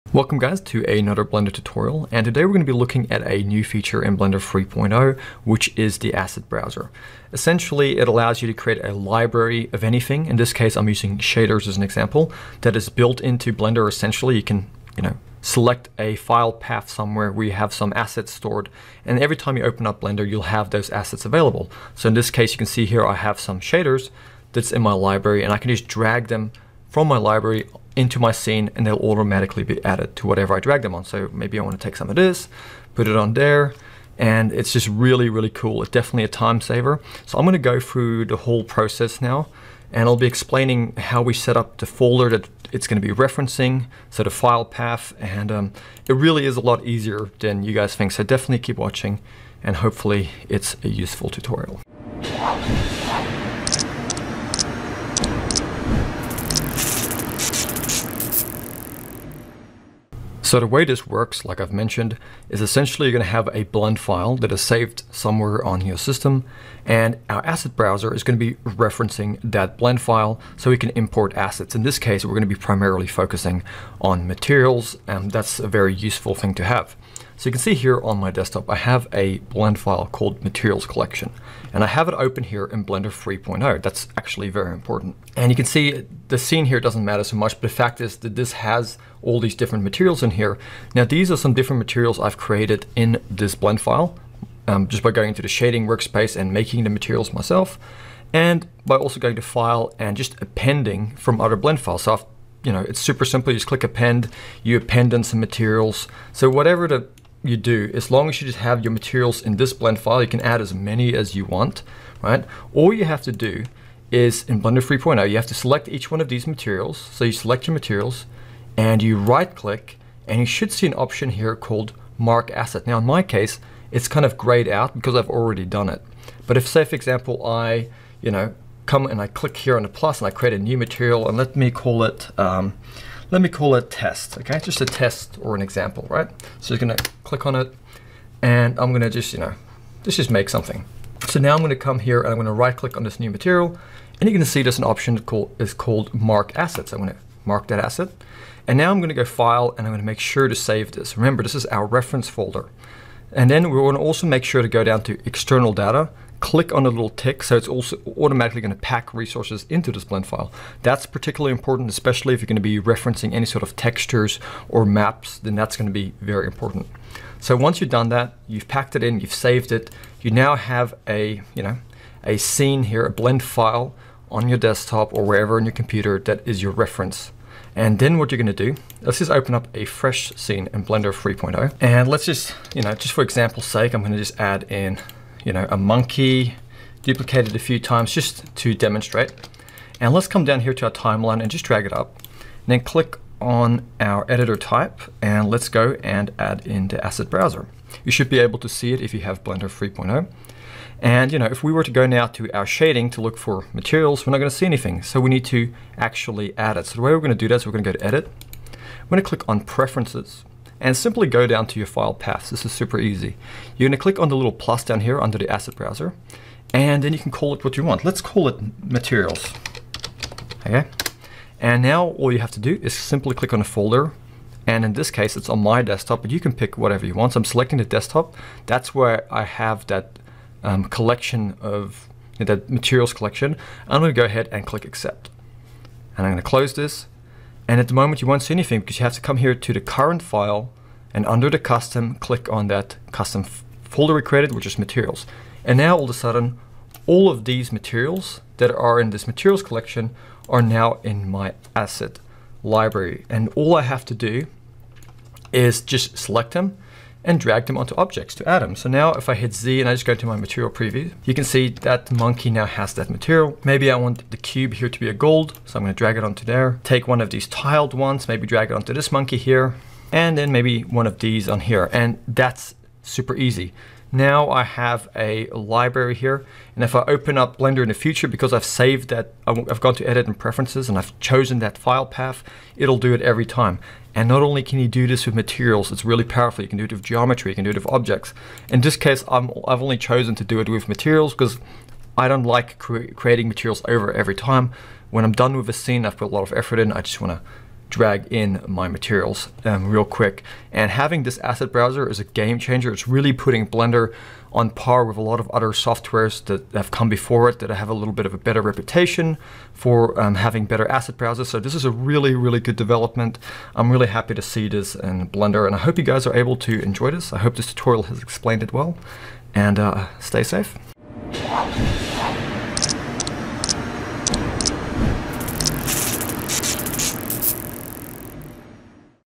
NEW Asset Browser Is Awesome | Blender 3.0
Use Blender’s asset browser to speed up your workflow.